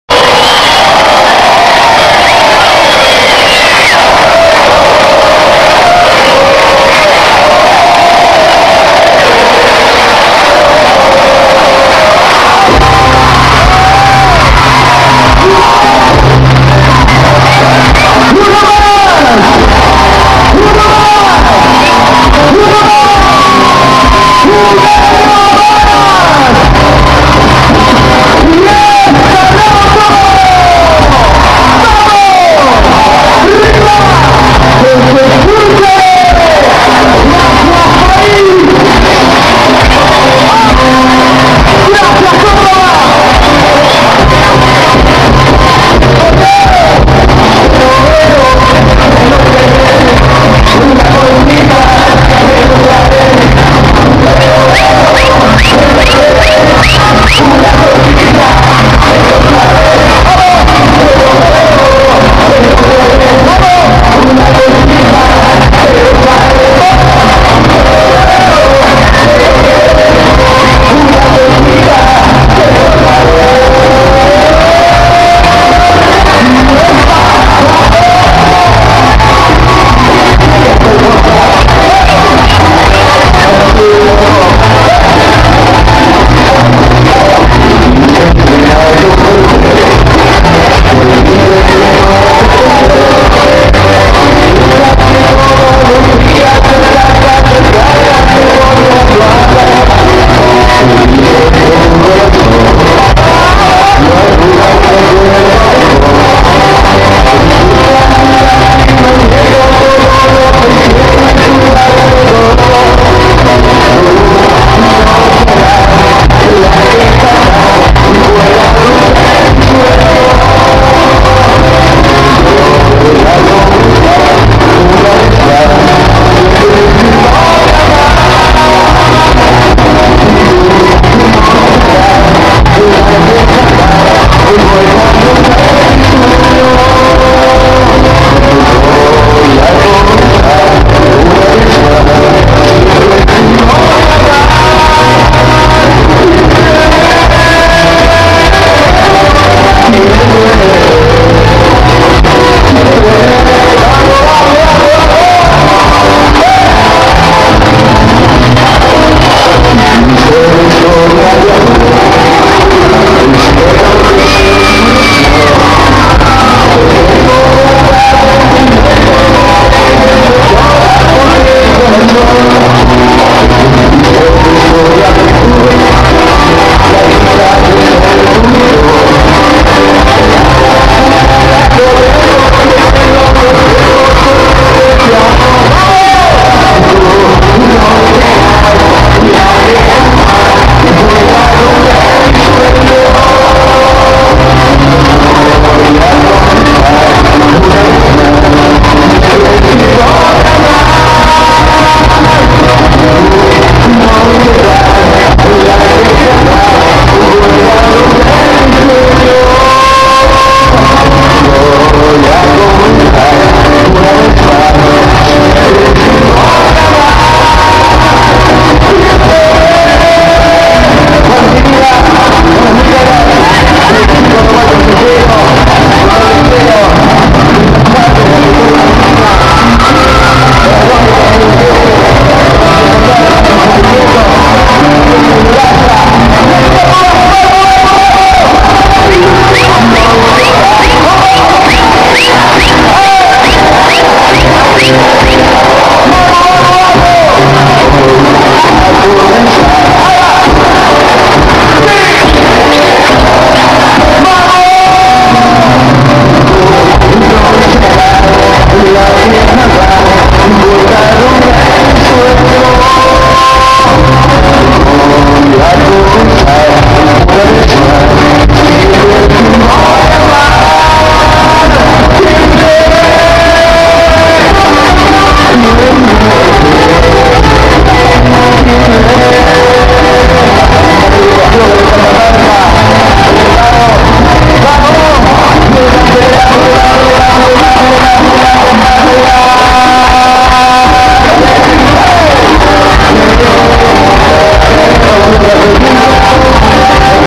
♫ ♪ 7° ANIVERSARIO [Parte 3].. ♪
Para revivir lo que fue el 7° Aniversario de La Fiesta.. Junto a Cristian Abel "EL LOCO" Amato.. En Vivo Desde FORJA..